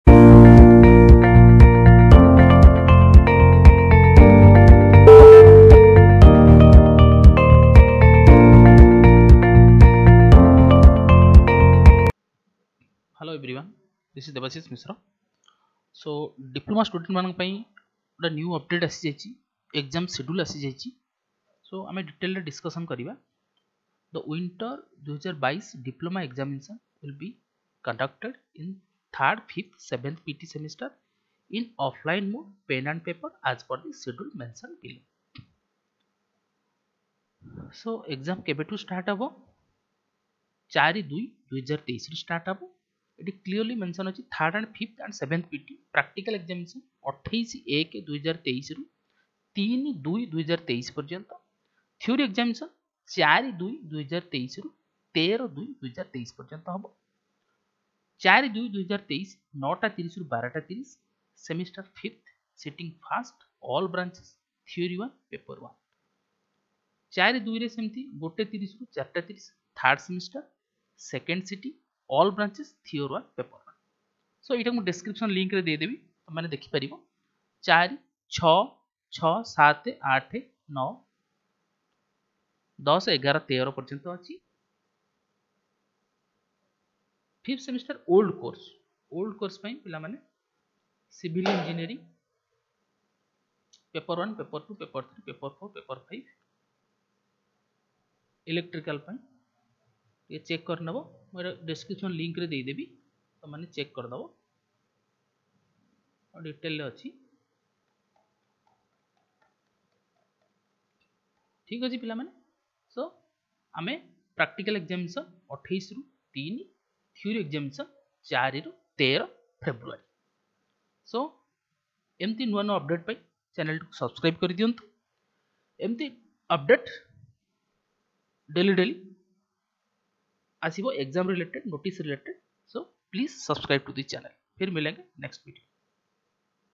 DIPLOMA EXAM SCHEDULE PUBLISHED.
In this Video You will Learn
About
1. EXPECTED EXAM DATE FOR 3RD AND 5TH SEMESTER DIPLOMA ??
2. EXPECTED EXAM DATE FOR 1ST SEMESTER STUDENTS ??
3. LAST DATE FOR CLASS?
4. DETAILS ALL ABOUT YOUR SYLLABUS.
5. DETAILS ALL ABOUT YOUR INTERNAL EXAM.